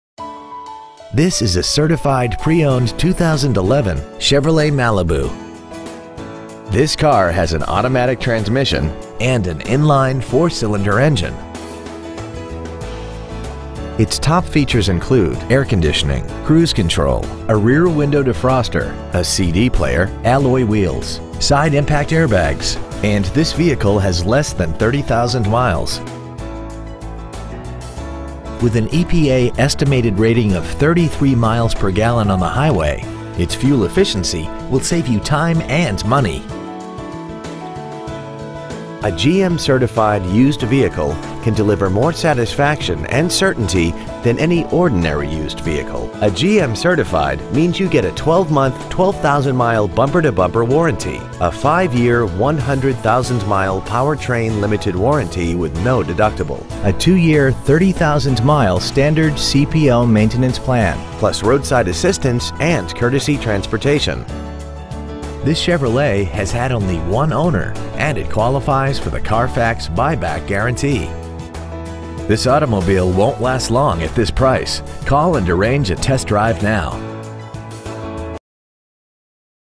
2011 Chevrolet Malibu Certified Chandler AZ 85225
Year : 2011
 Make : Chevrolet
 Model : Malibu
 Engine : 2.4L 4 Cyl. Fuel Injected
 Trans . : Automatic
 Exterior : White
 Miles : 29,342

 Stock : 17625

 2121 N. Arizona Avenue